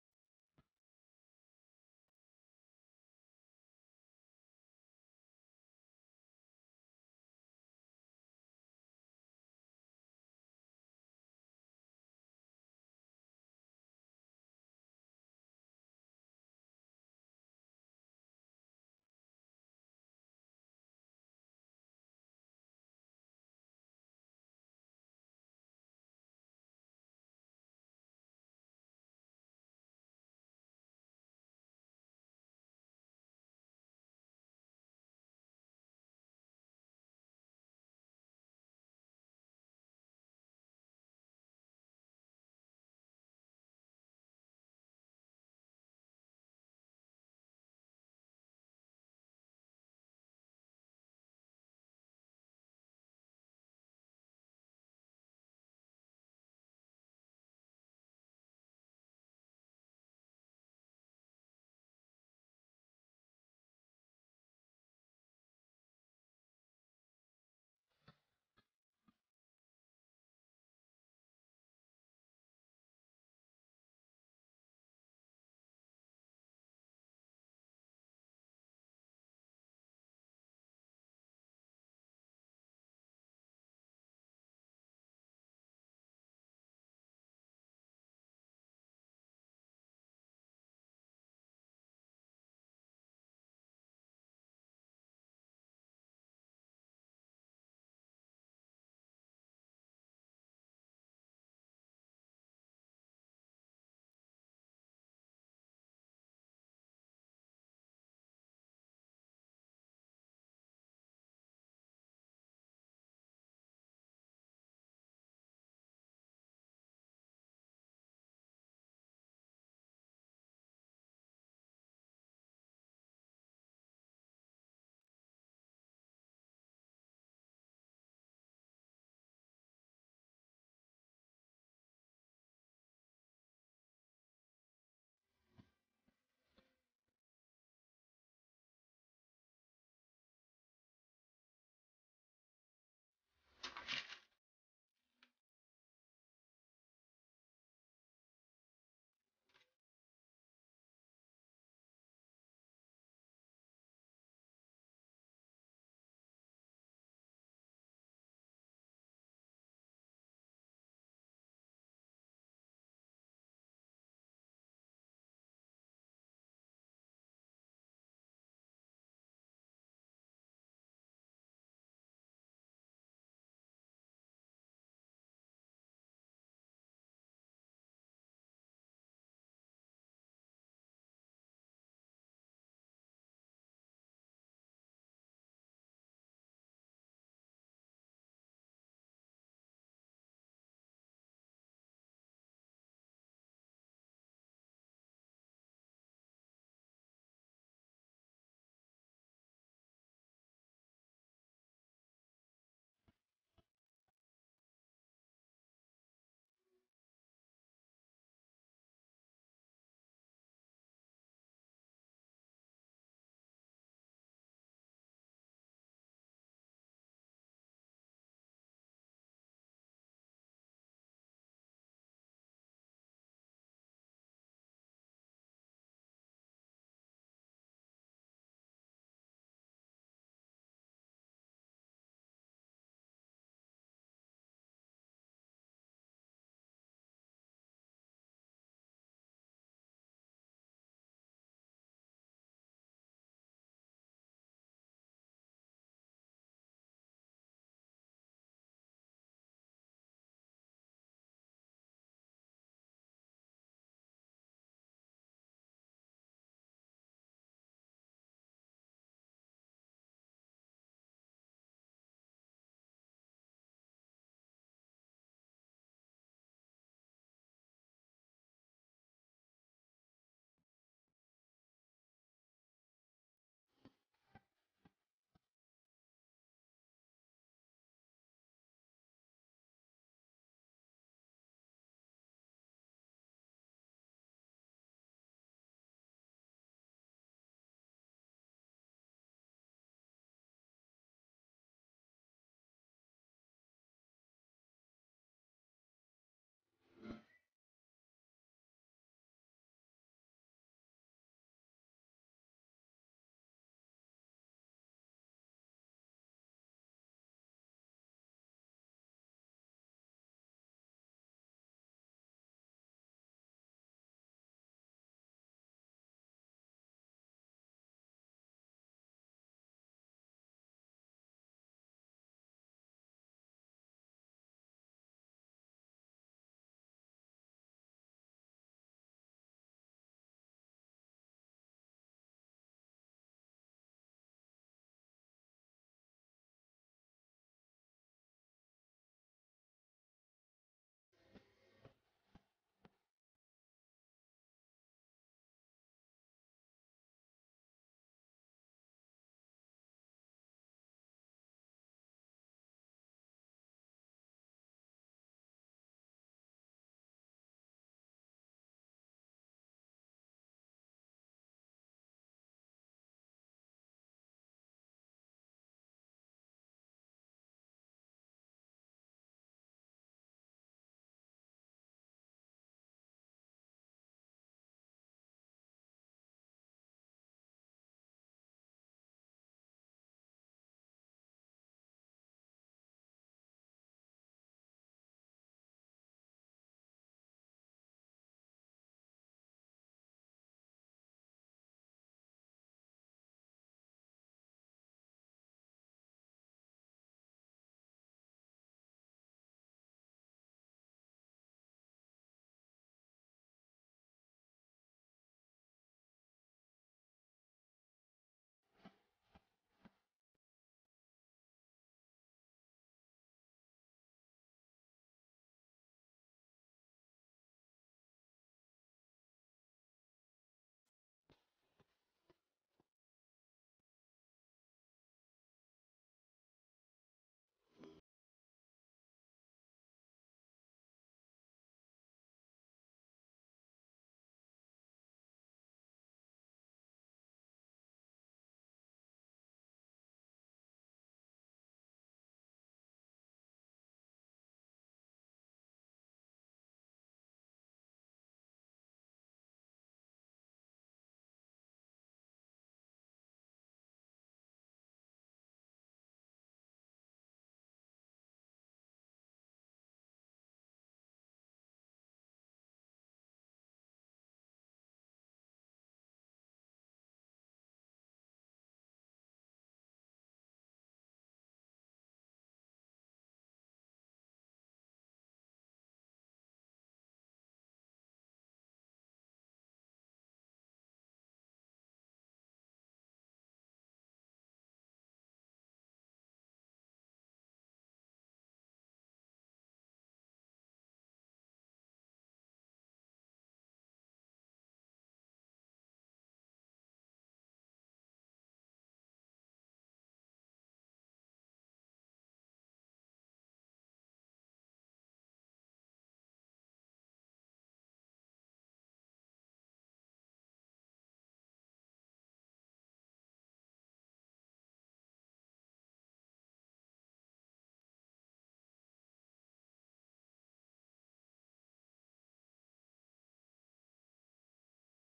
Scratchboard Drawing of Hops for Michelob Amber Bock
This is a time-lapse video showing how the hops that are featured on Michelob Amber Bock were drawn. The drawing is drawn by hand on scratchboard by Michael Halbert.